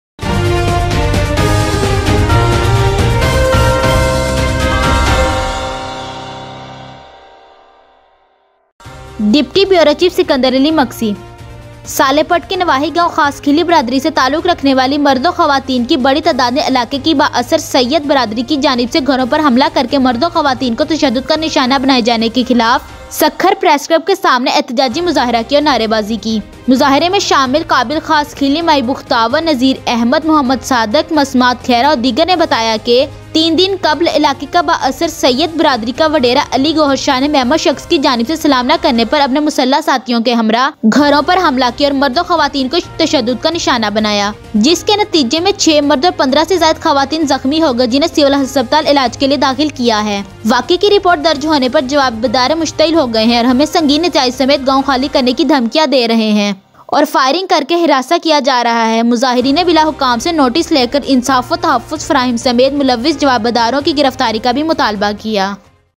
Men and women protested in front of Sukkur Press Club against being targeted
Video from RYK Live صالح پٹ کے نواحی گاؤں خاصخیلی برادری سے تعلق رکھنے والے مرد و خواتین کی بڑی تعداد نے علاقے کی بااثر سید برادری کی جانب سے گھروں حملہ کرکے مرد و خواتین کو تشدد کانشانہ بنائے جانے کے خلاف سکھر پریس کلب کے سامنےاحتجاجی مظاہرہ کیا